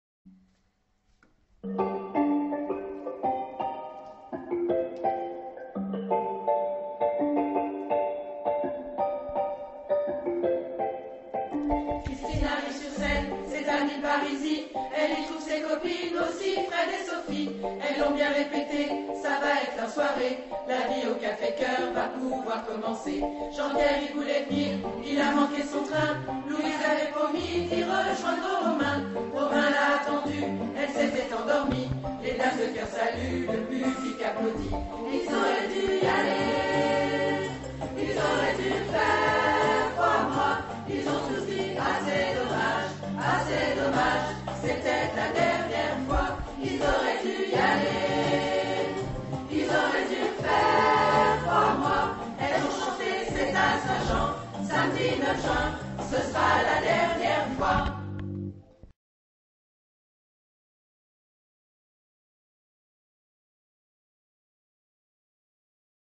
Dames de Choeur à Saint Jean
Au café Chœur, avec les Dames de Chœur vous vous laisserez séduire par cette comédie musicale originale, pleine d'humour et de tendresse. Avec les mots de Roger Davau, les personnages s’animent pour interpréter avec Fred le pianiste, les plus belles chansons.